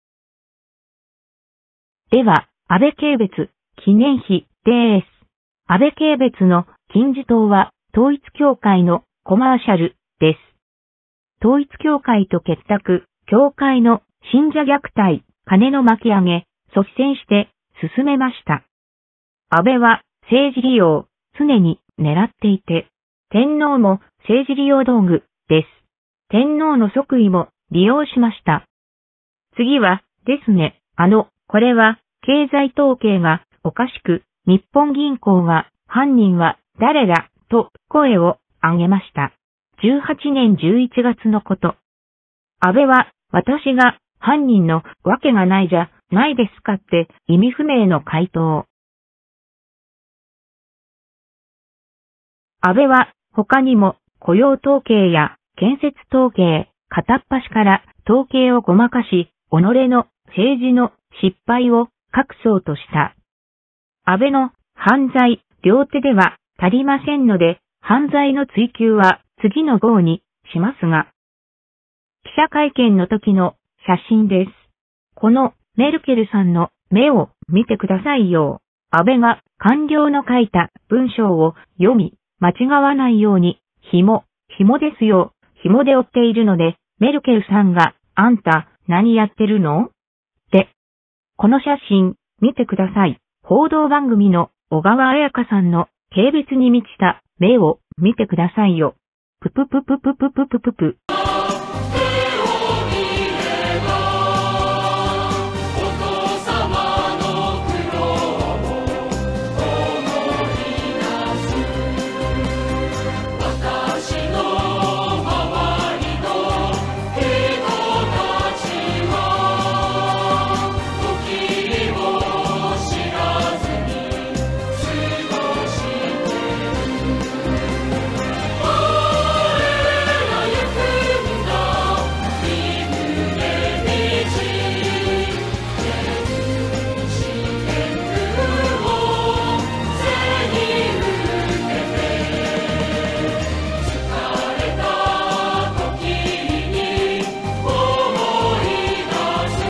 安倍チン三軽蔑の記念碑、国葬儀に先立ち公開。チン三の思い出に浸りましょう。
軽蔑シーン写真などが多数で、１本のYou Tubeでは足りません。